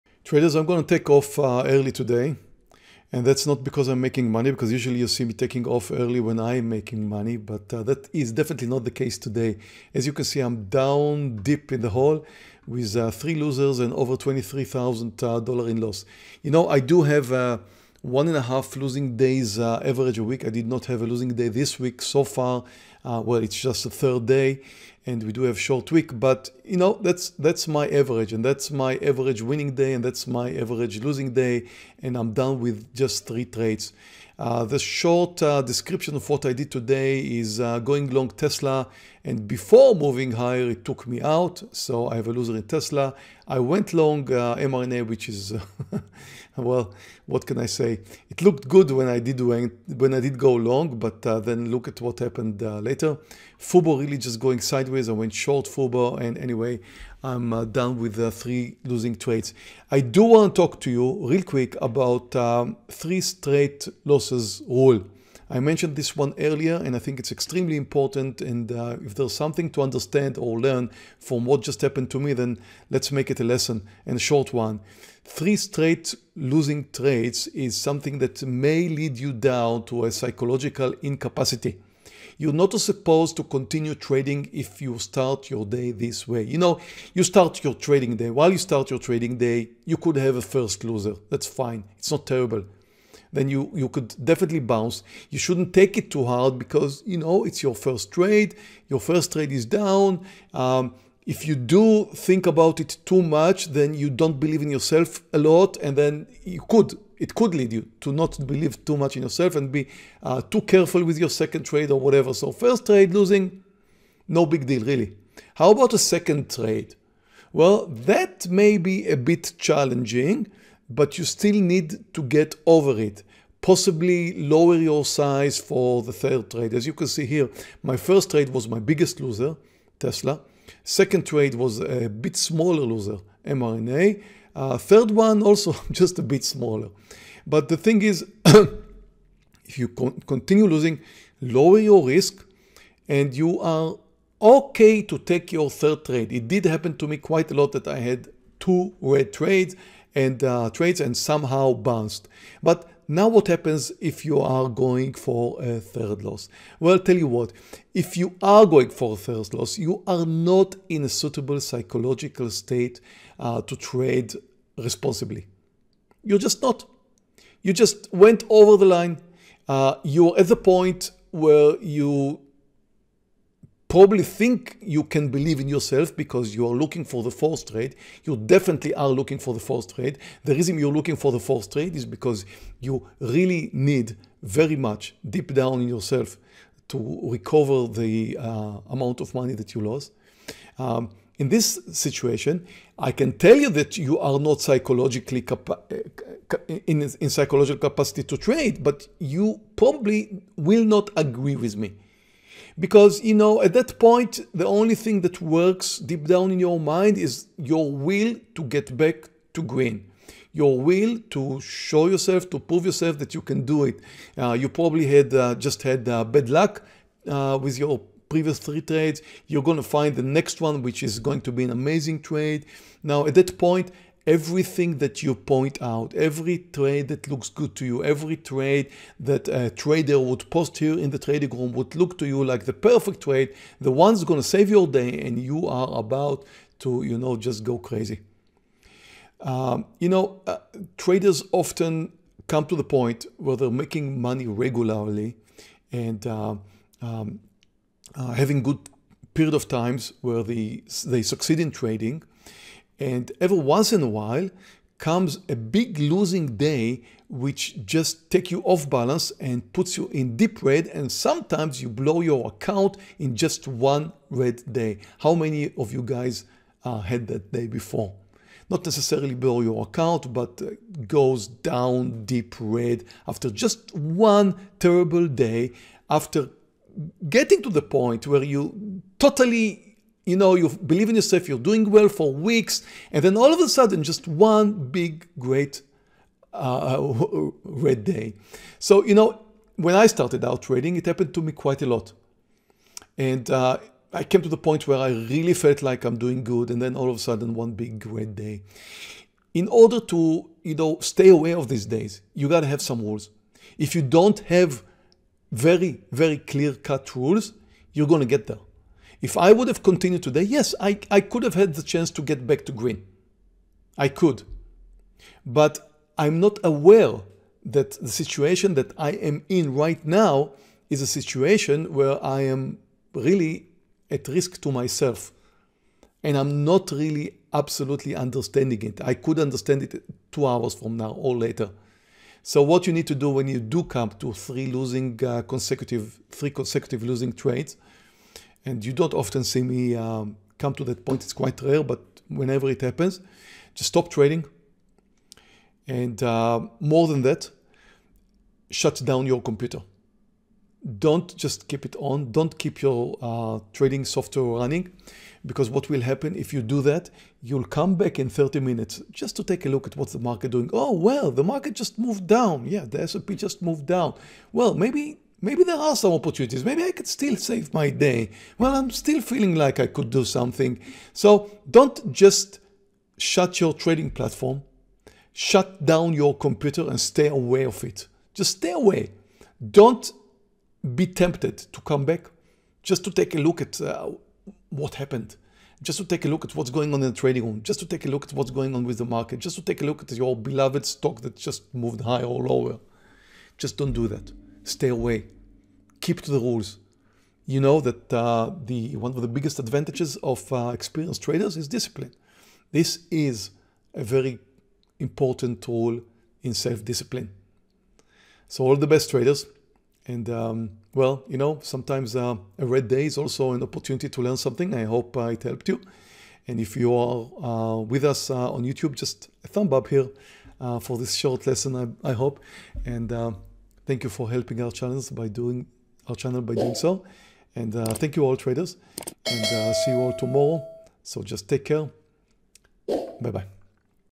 The Three Strike Rule (Day Trading)
🎁 Special holiday prices across all of Tradenet's programs!

Live Day Trading Stocks - Losing $23,000 trading FUBO, MRNA and TSLA. Meir Barak live day trading on December 30th 2020.Three strike rule...